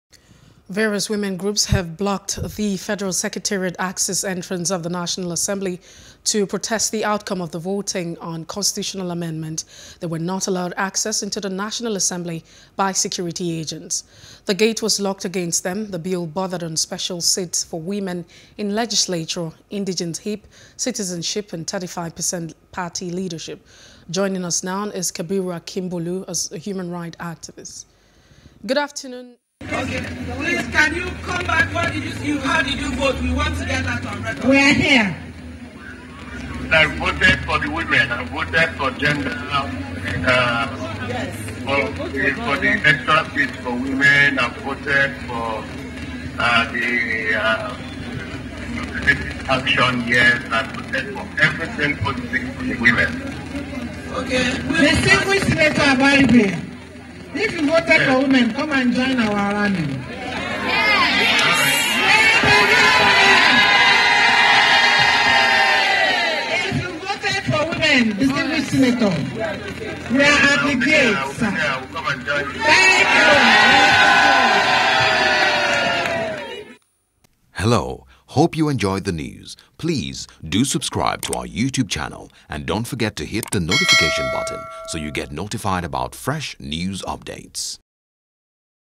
Constitution Review: Protesters Block NASS Gate Over Rejected | NEWS
Various women groups have blocked the Federal Secretariat axis entrance of the National Assembly to protest the outcome of the voting on constitutional amendment.